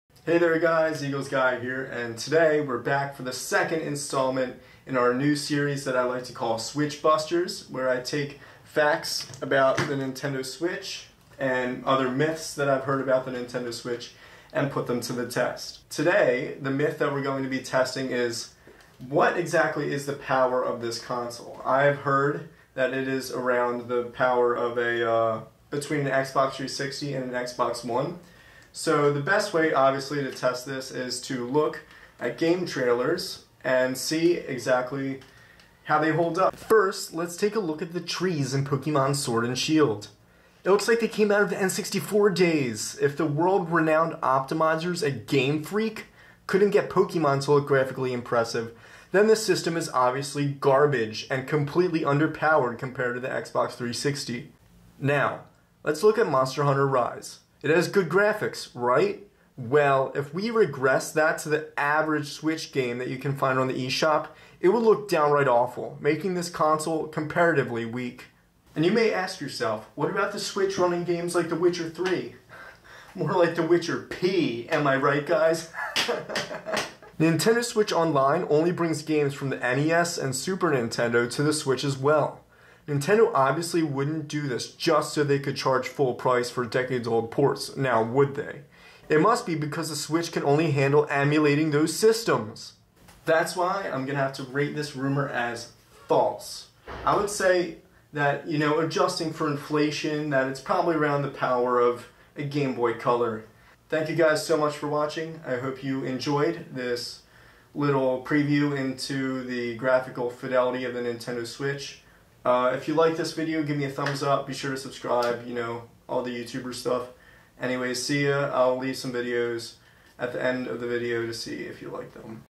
WEAKER Than I Thought? How POWERFUL Is The Nintendo Switch?
How powerful, graphically speaking, is the Nintendo Switch? Today, we're going to find out in the most scientific way possible... By watching game trailers. I have heard that it is approximately as strong as the Xbox 360 in handheld and stronger in docked, but we will see...

Do you have any Switch Rumors that you want me to test? Write them in the comments!

EaglesGuyTV